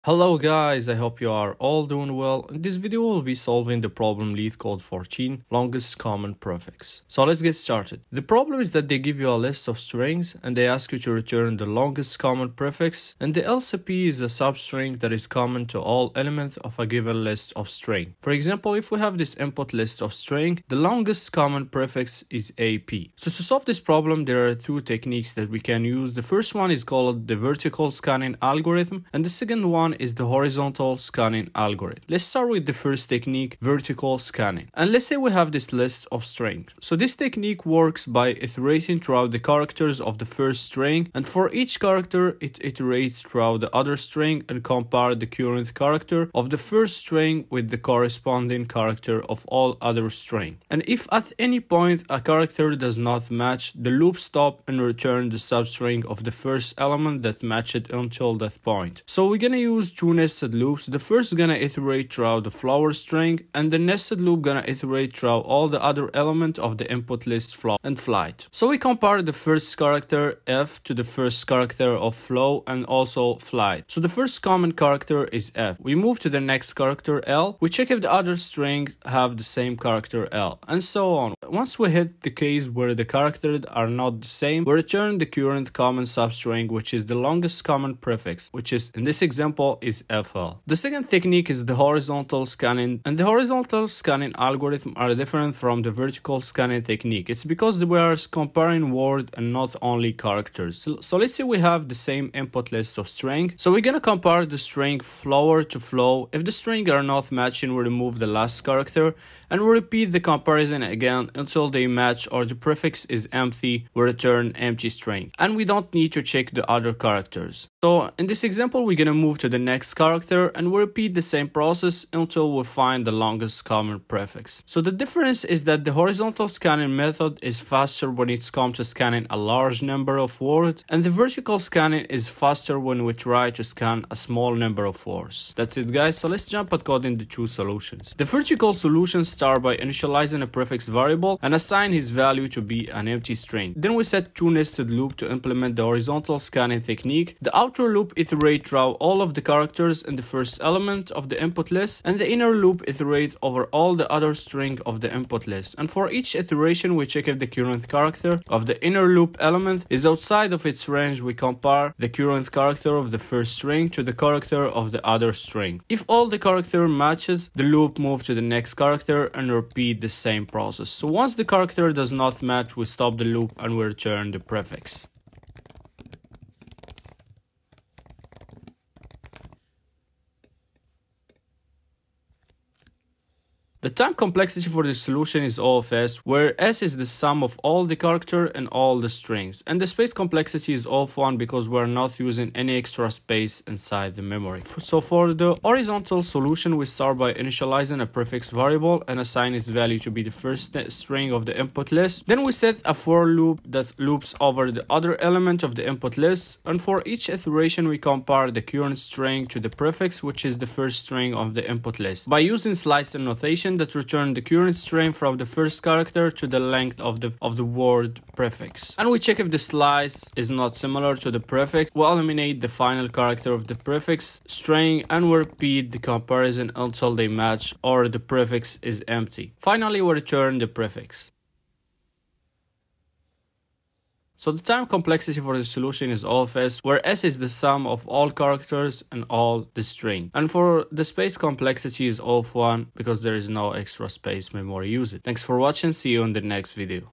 Longest Common Prefix - LeetCode 14 - Coding Interview Questions
The problem is that they give you a list of strings, and they ask you return the longest common prefix, and The LCP is a substring that is common to all elements of a given List of strings.

To solve this problem there are two techniques that we can use, The first one is called the Vertical scanning algorithm, the second one is the Horizontal Scanning Algorithm.

Next, we present the solution and walk you through the code step by step, highlighting the key points and explaining the logic behind each part of the solution.

Whether you are preparing for a technical interview or just looking to improve your coding skills, this video is a great resource.

Reading The problem : (0:00)
Vertical Scanning Solution : (0:23)
Horizontal Scanning Solution : (1:31)
Vertical Code Explanation. : (2:15)
Complexity Analysis : (3:03)
Horizontal Code Explanation : (3:14)
Complexity Analysis : (3:54)